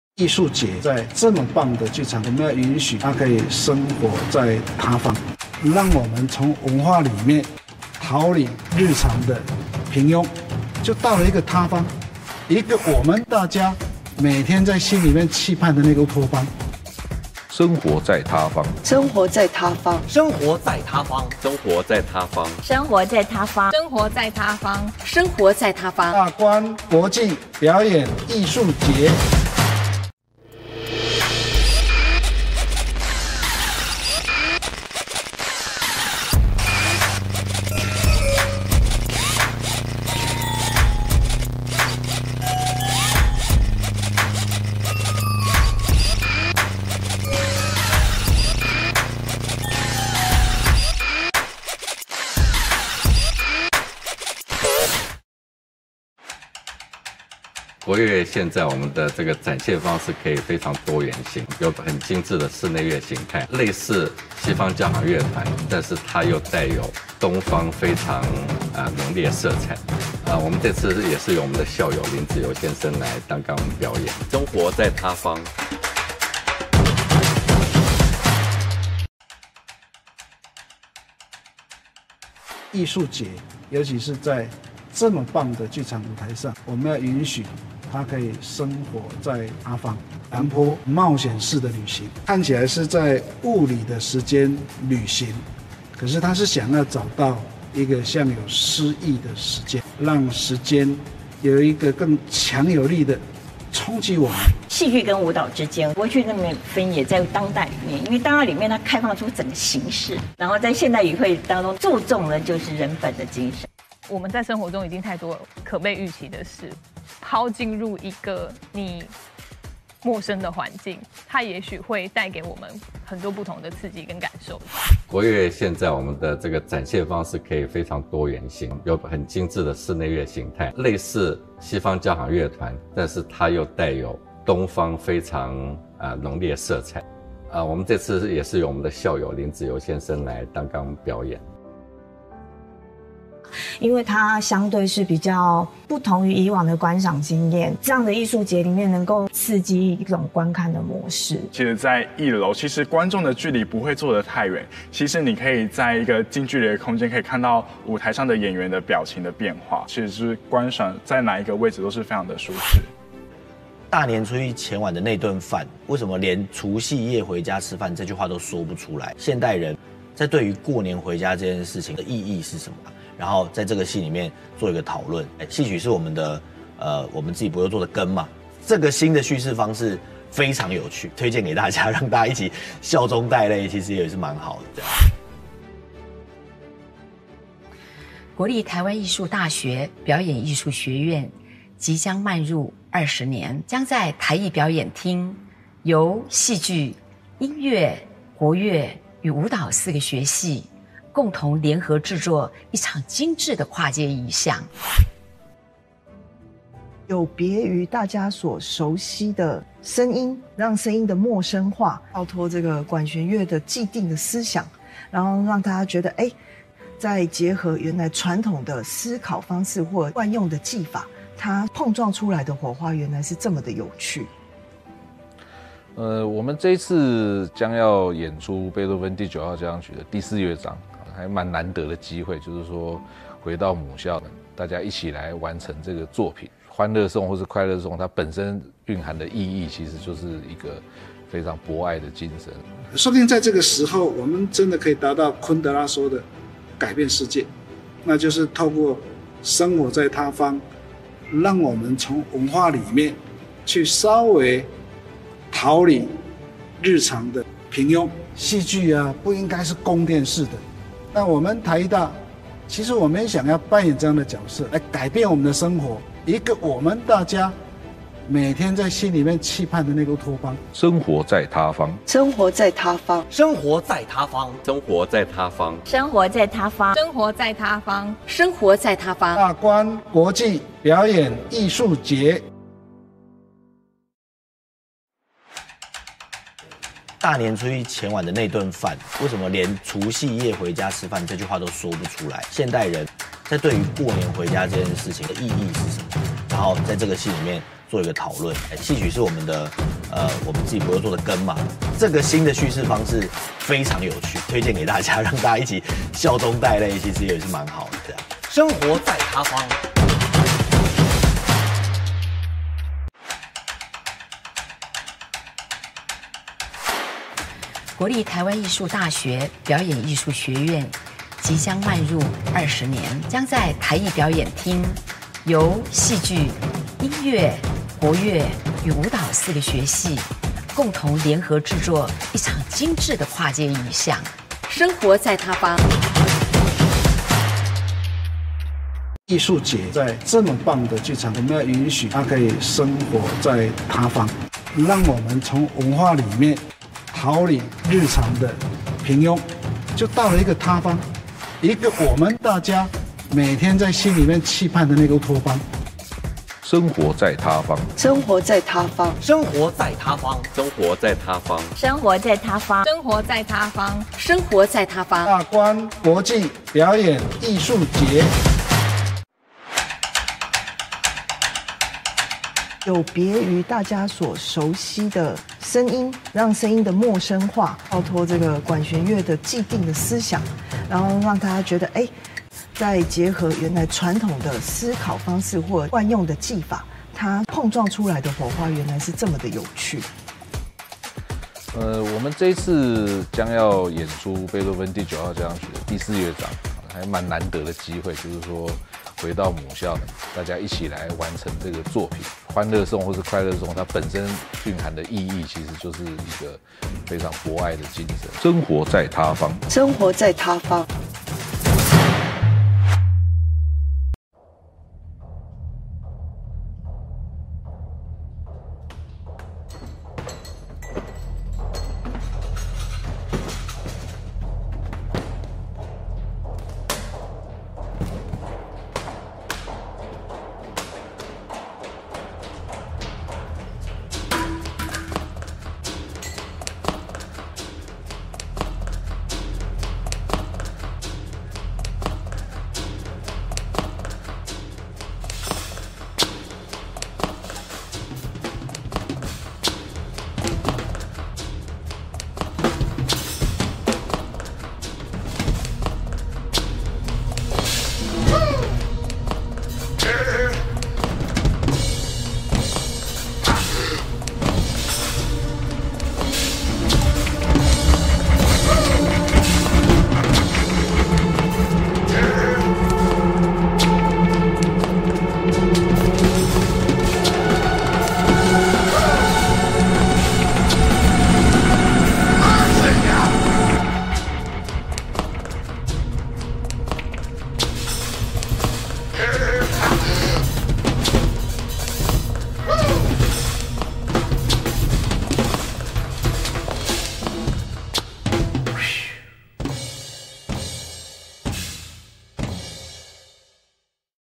2020大觀國際表演藝術節《生活在他方》
在米蘭．昆德拉（Milan Kundera, b.1929-）的小說世界裡，寫成一部青春的史詩，深刻描繪一位詩人生命中的愛與叛逆、理想與現實的悲劇衝突。在現實世界裡，生命在承受疫情衝擊的影響下，無法伸展與暢活。雖然環境不是自由的，但我們的想像力可以是。

「假如我們不能改變這個世界，那我們至少應該改變我們的生活，自由自在地活著」（──米蘭．昆德拉），今年邀請以戲曲重新詮釋現當代議題而知名的栢優座《大年初一前晚的那頓飯》，如何貫穿古今，從失落走向心中最柔軟的「他方」；大觀舞集《搖擺.D》自4 位風格迥異的編舞家共同翻轉現代舞，思索身體如何在時空中飄移而找到自在之所；力邀傑出校友重磅回歸，由臺藝大自製節目《國樂萬花筒》、《跨界藝象》、《藝韻薪傳》，藉表演藝術華麗的喧囂與奔放，在心靈自我放逐，在意識流動的過程中不斷自問自答。大觀國際表演藝術節「生活在他方」佐詩歌與你攜手抗疫，追逐心中的烏托邦。